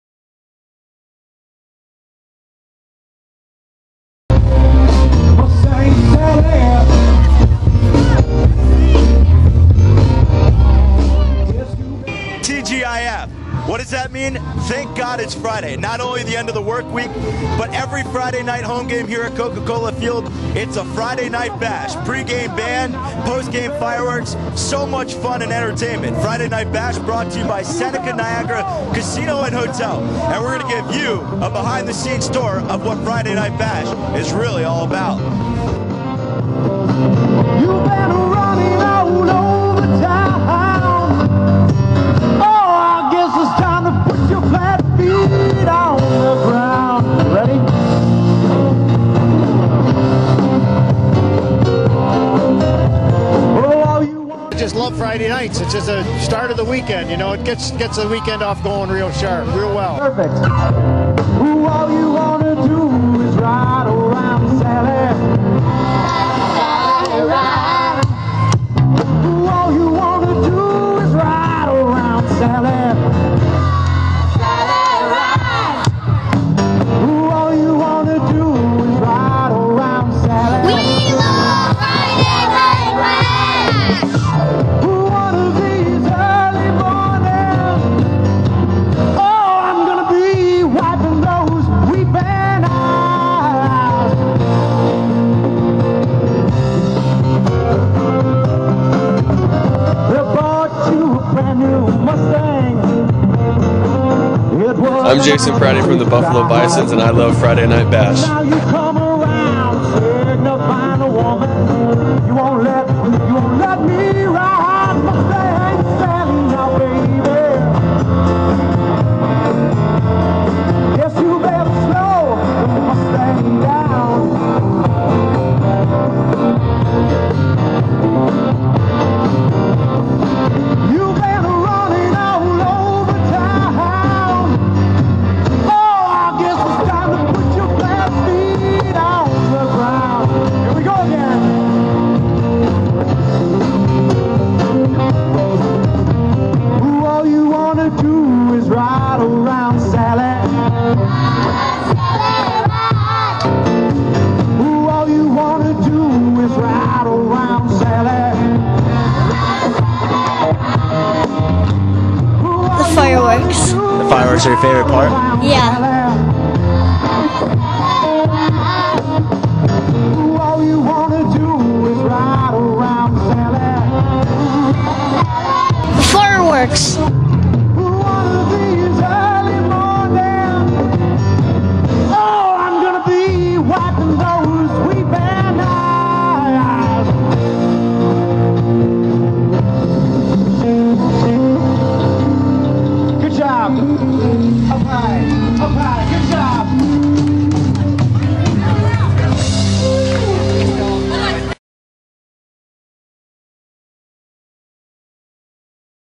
6/15 -fridaynightbash! feature
The best night of the week is always a Buffalo Bisons fridaynightbash!, presented by Seneca Niagara Casino & Hotel. With a pre-game plaza party and post game fireworks, there is something for every fan at Coca-Cola Field on Fridays.